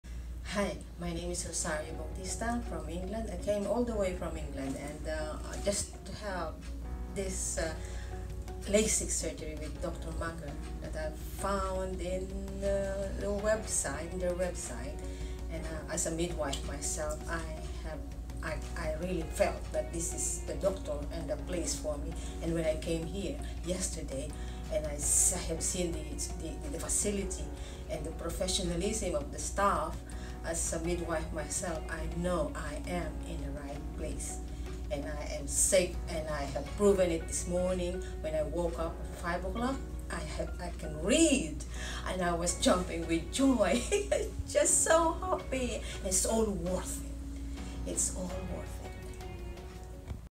Rosario comes all the way from London to California to have LASIK Eye Surgery!
Listen to Rosario's testimonial about her LASIK experience at Dr. Manger's Office, Saddleback Eye Center.

Saddleback Eye Center
Laguna Hills, CA